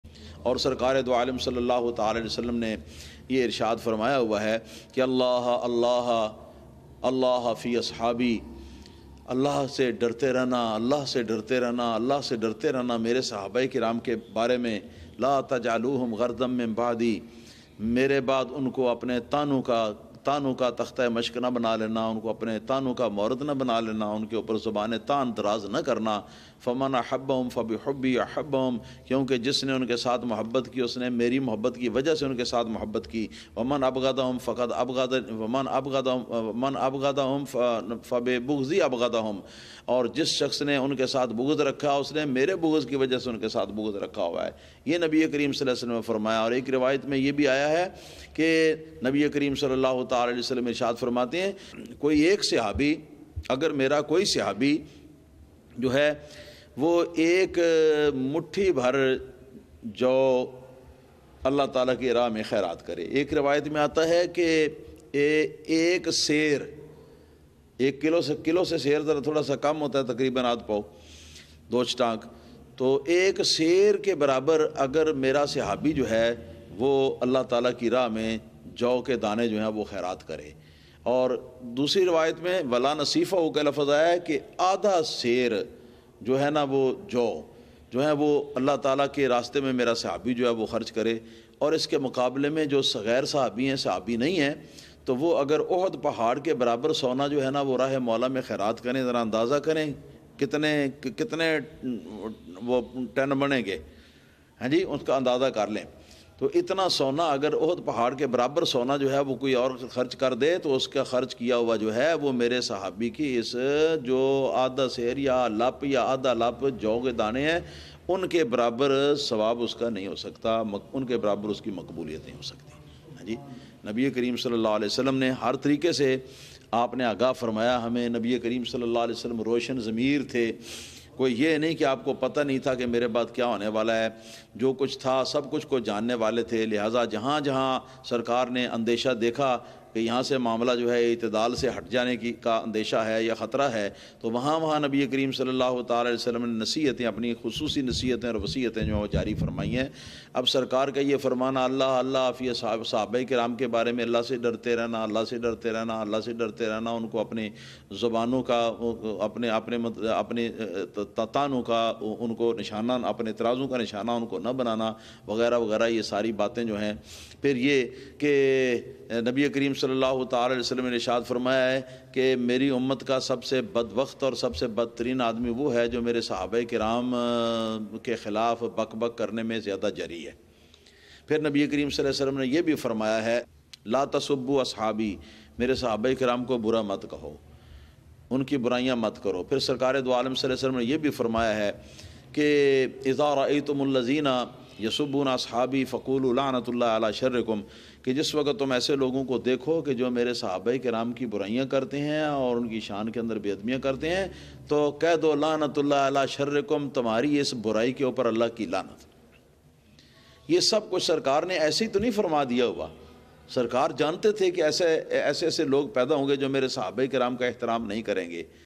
Farman E Rasool | Allah Se Darte Rehna | Mere | Sahabi ke Bare Main | Fatwa Tv
Muftiye Azam Pakistan Mufti Muhammad Ashraf-ul-Qadri Muhaddas Naik Abadi Sajjada Nashin Murarian Sharif(Naik Abad)
Bani Al-Jamia -tul-Ashrfia Ali Masjid Markazi Gujrat Pakistan